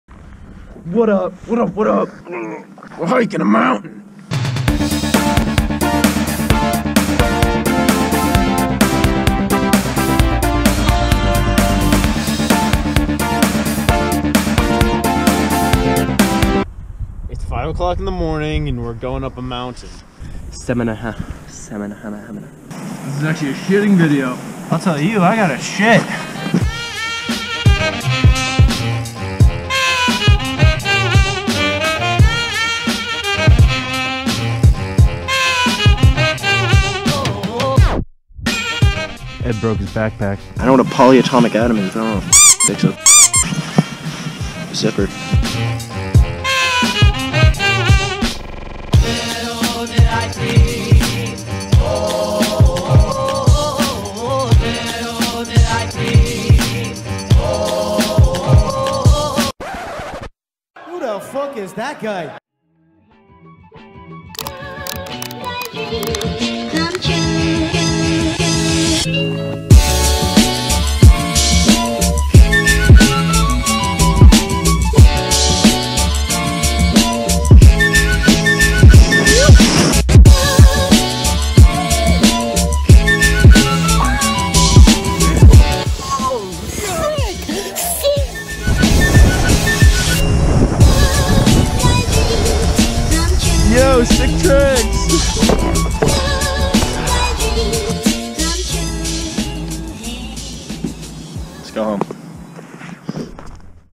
MOUNTAIN MORNINGS
We got up early to hike a mountain and snowboard down. Tune in and enjoy!